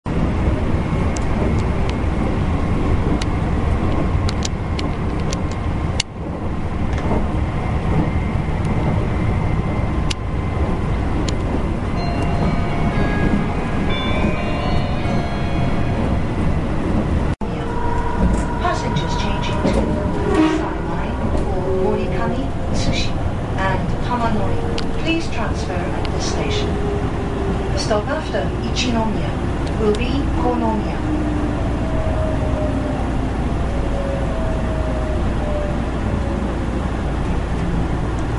名鉄２２００系 車内液晶モニター エラー文字！！
岐阜から名古屋まで、エラー文字が出ていました